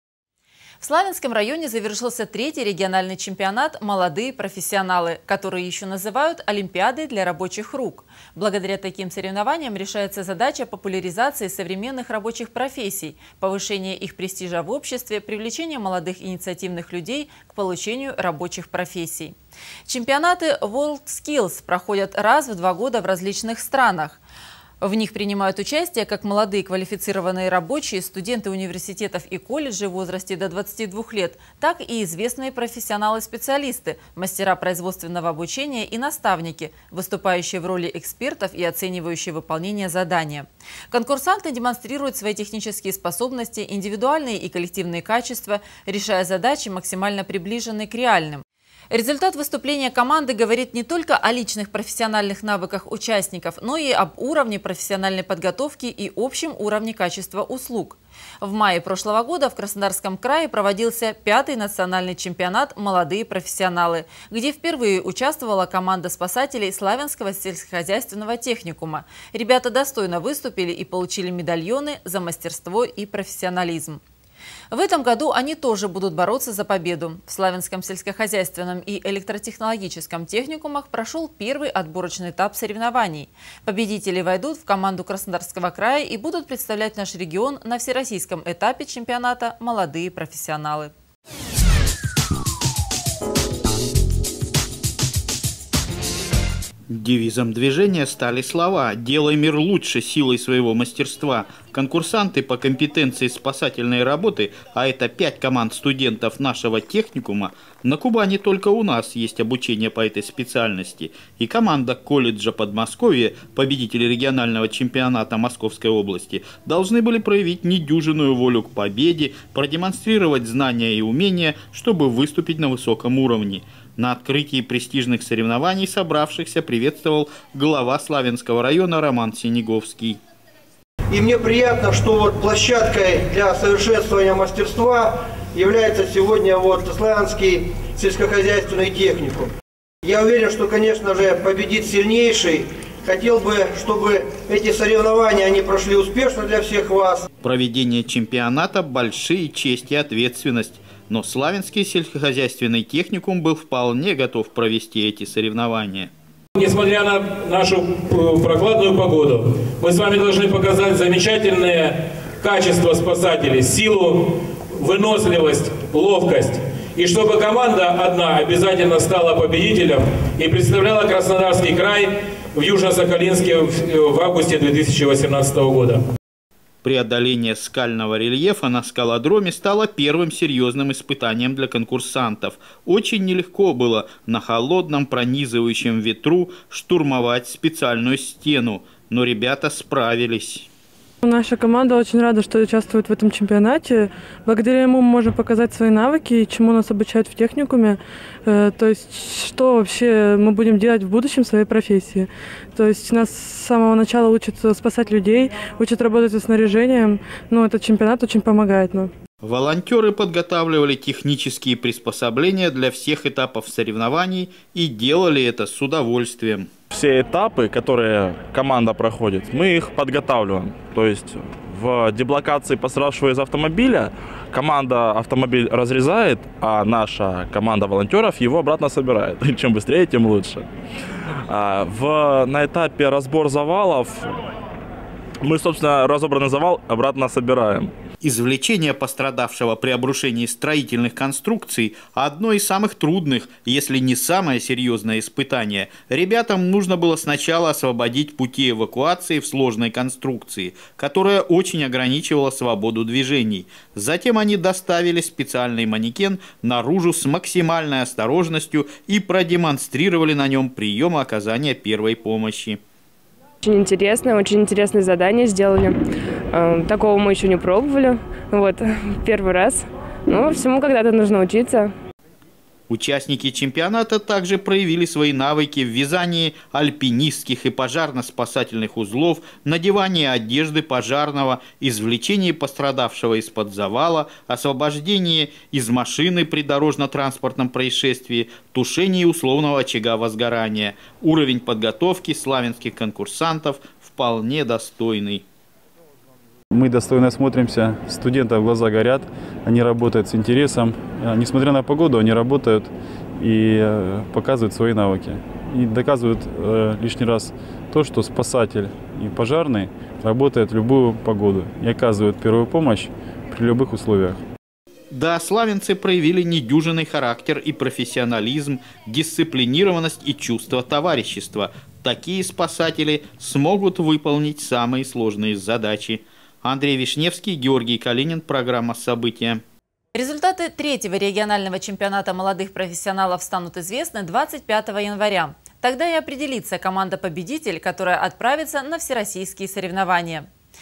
В городе завершился 3 региональный чемпионат «Молодые профессионалы»
В Славянском районе завершился 3-й региональный чемпионат «Молодые профессионалы», который еще называют  «олимпиадой для рабочих рук». Благодаря таким соревнованиям  решается задача популяризации современных рабочих профессий, повышения их престижа в обществе, привлечения молодых инициативных людей к получению рабочих профессий.